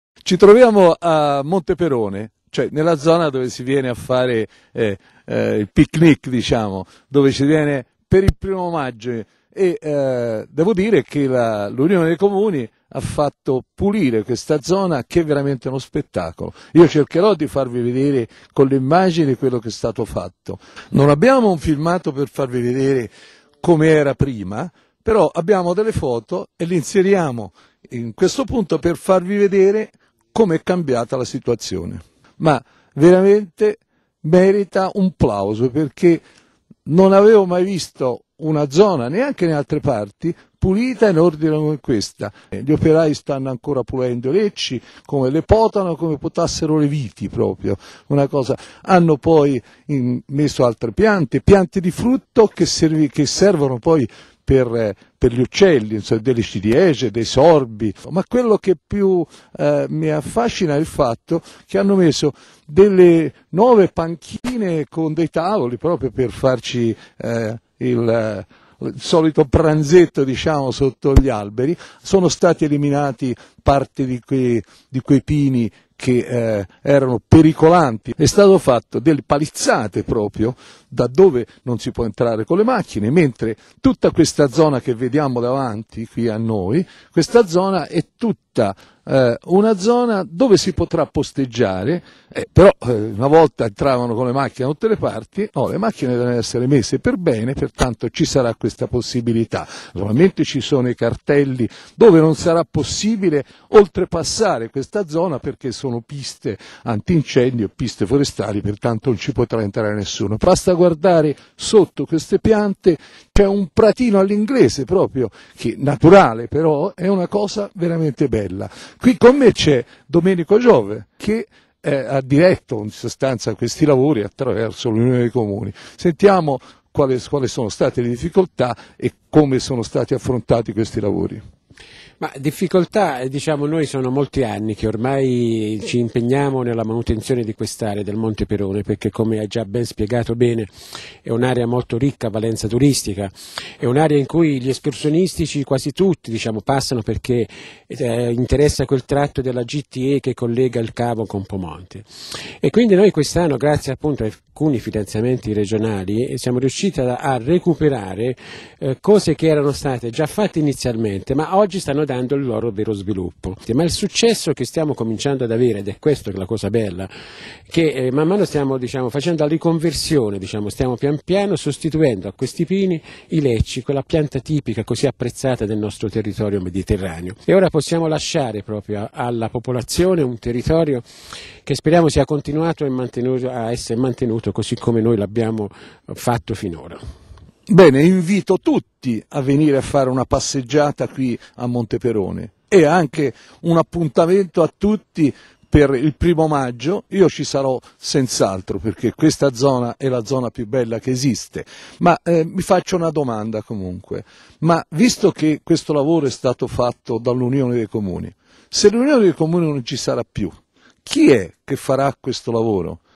Ripristinata l'area pic nic di Monte Perone
Intervento delle squadre forestali dell'Unione di Comuni: bonificata la zona, sostituiti pini pericolanti con lecci, aggiustate e sostitiute la sedute, riorganizzati i parcheggi